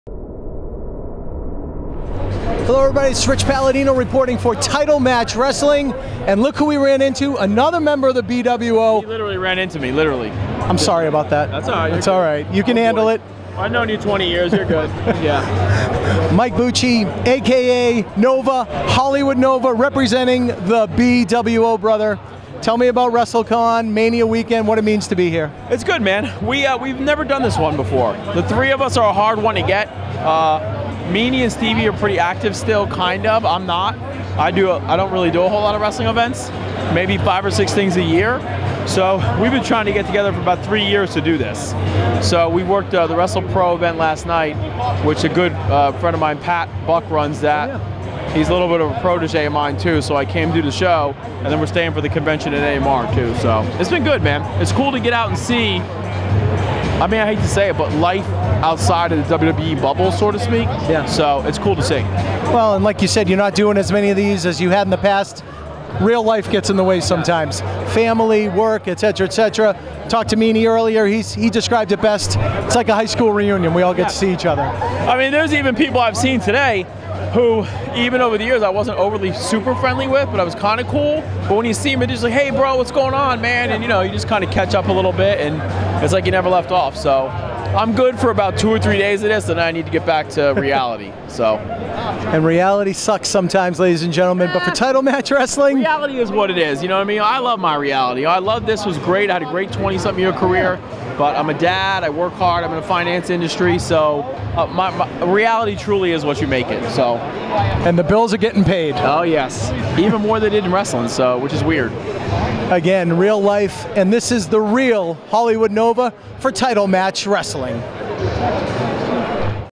Mike Bucci (Simon Dean, Nova) Interview | Wrestlecon 2017
You know him as Nova in ECW and Simon Dean in WWE! Our cameras caught up with Mike Bucci at Wrestlecon in Orlando last weekend. Host Rich Palladino talked with Bucci about his Wrestlecon experience, upcoming events and more. This clip was shot by Worked-Shoot Productions. Follow their awesome work